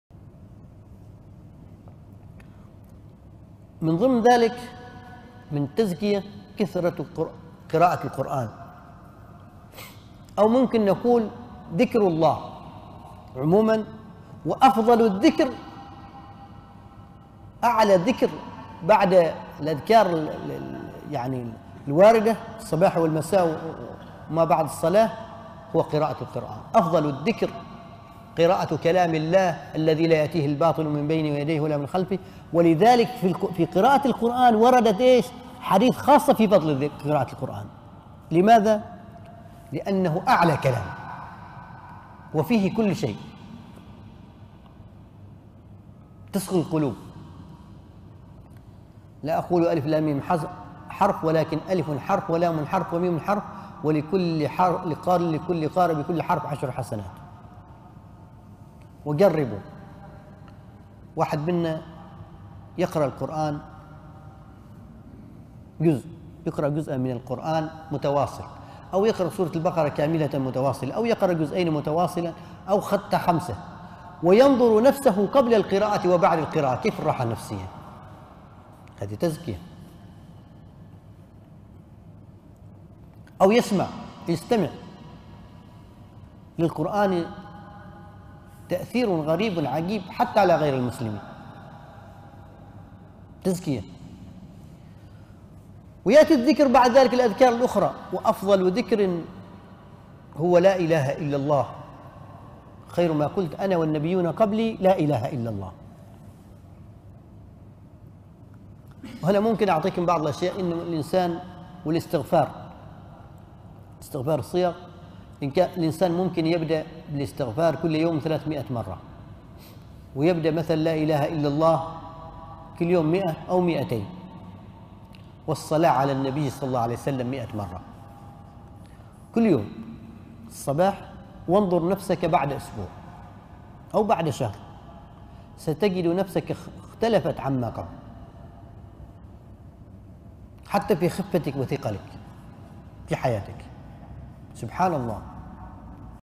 بذكر الله تجد الفرق الشاسع تصقل القلوب وتزهو الحياة وتزكي النفوس. للشيخ عبدالله الهنائي
مصدر المقطع:

درر نورانية من قواعد قرآنية [2]

للشيخ عبدالله الهنائي